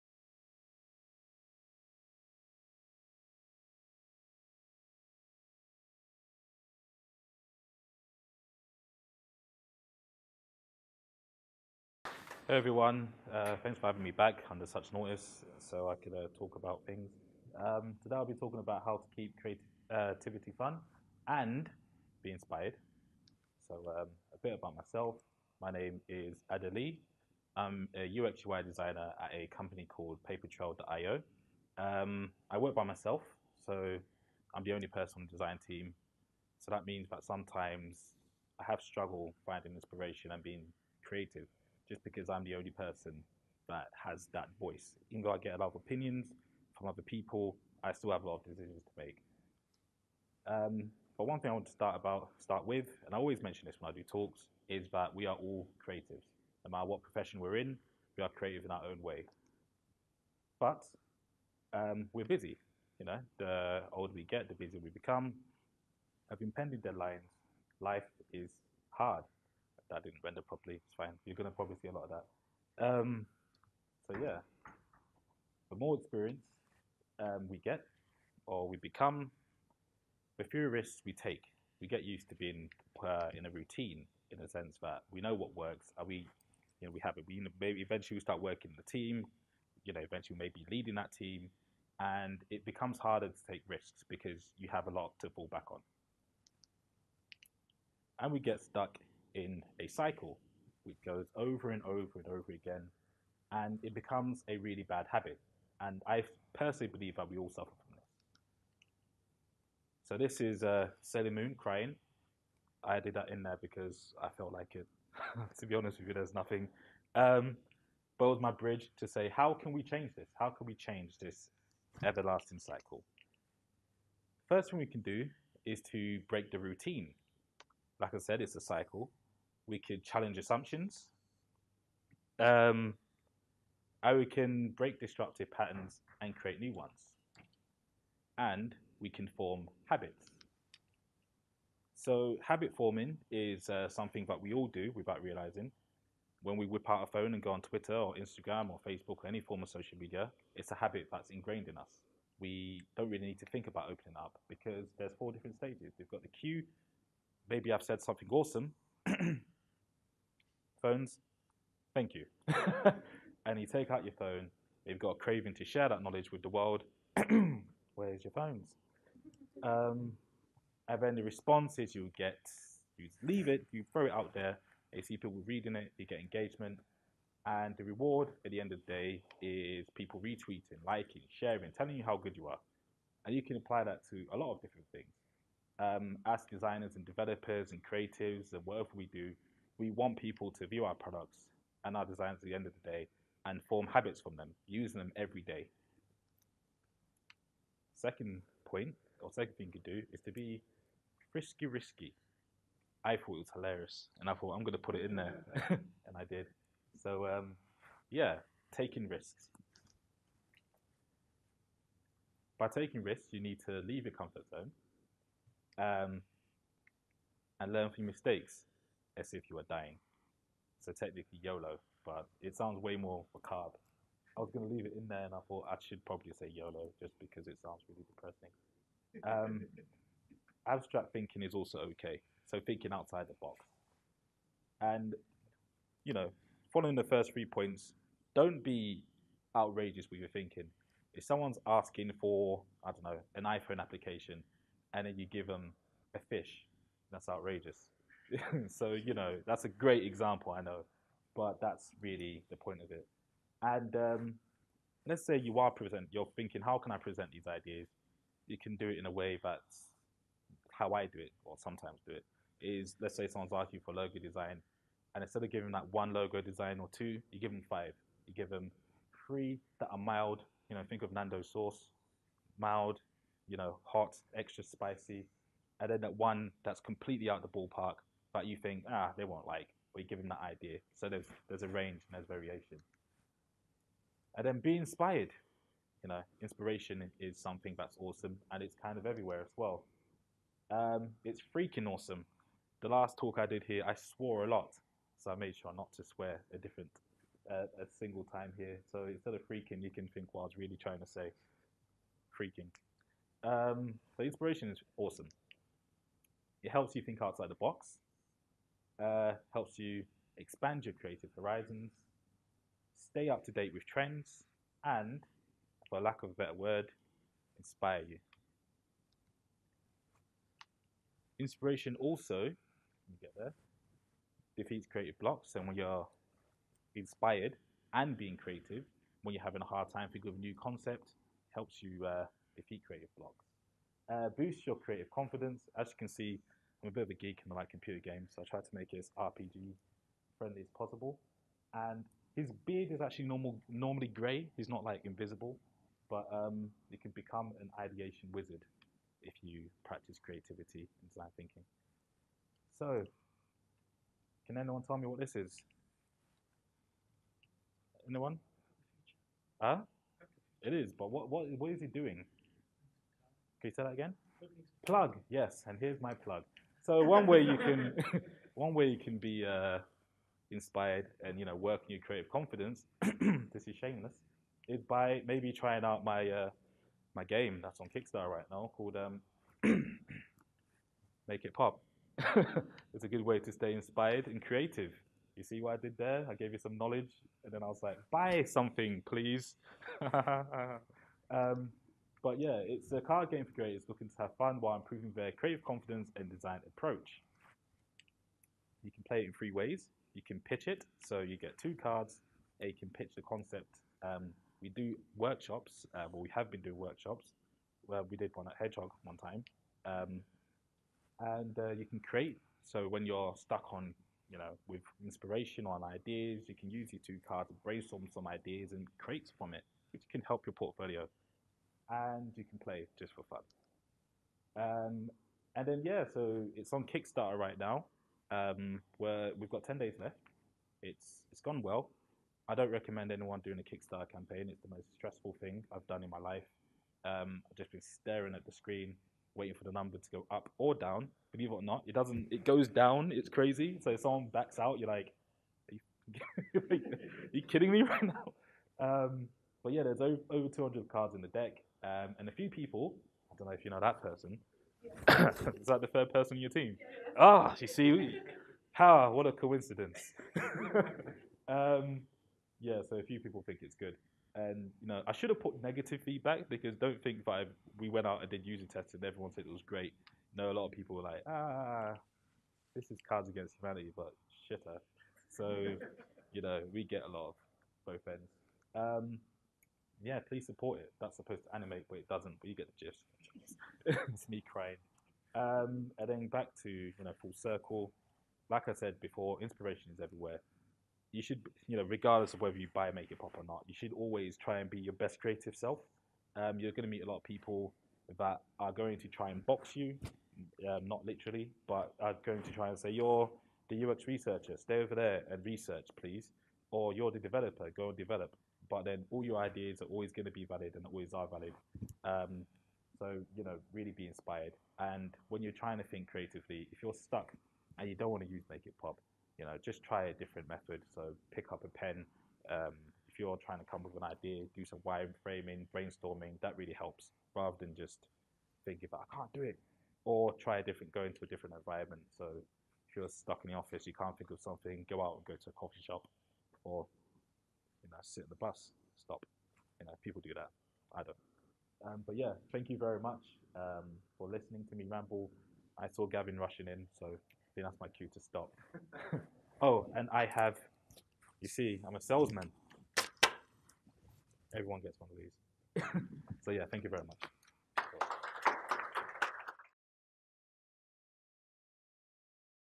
How to Keep Creativity Fun | Ade-Lee Adebiyi | Sunderland Digital September 2018
Ade-Lee Adebiyi, Lead UX/UI Designer at Papertrail.io, shows us how to keep creativity fun and be inspired to take risks.